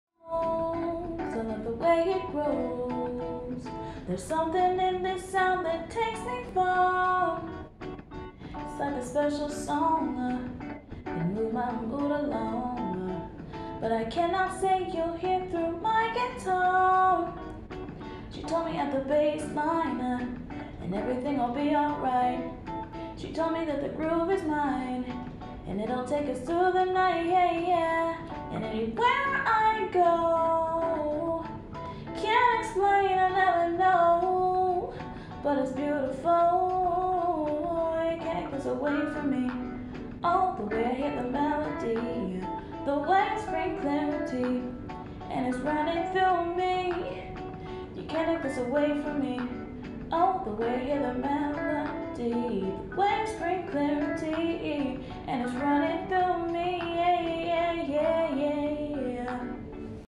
Quarantine Cover: Tom Misch
During this COVID19 pandemic, I decided to keep my artistic juices flowin' by making watercolor paintings and covers of some of my favorite musical artists. This one is of Tom Misch, an AMAZINGLY talented musician who I stumbled upon only by chance. (My work computer has a Youtube restriction on music and he was one of the few artists that made the cut).
Enjoy the painting and cover!